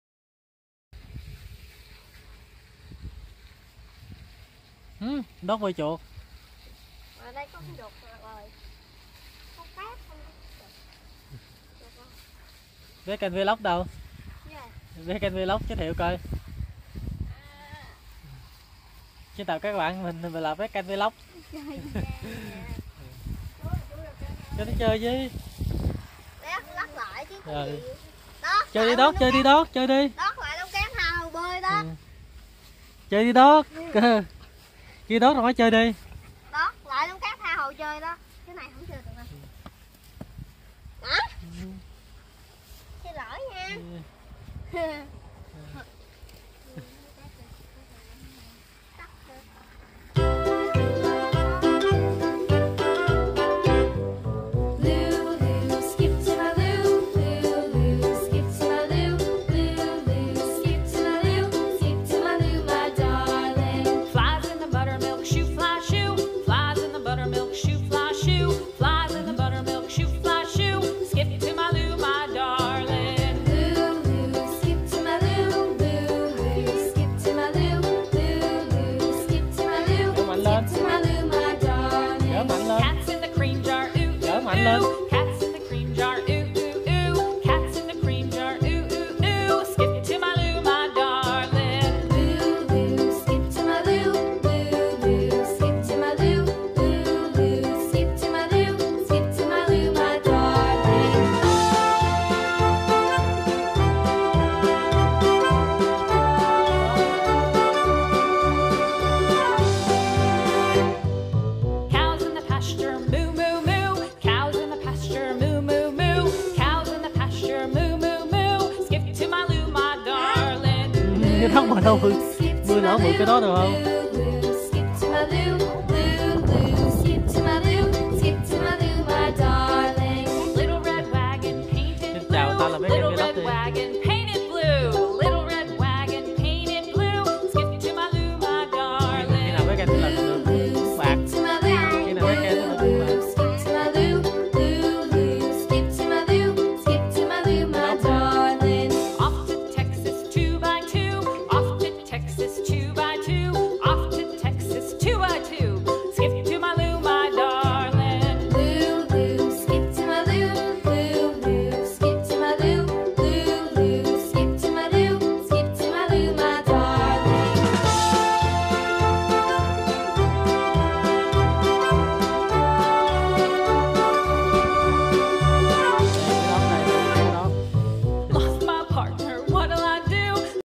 This dog is so cute
The video captures the moment the dog is playing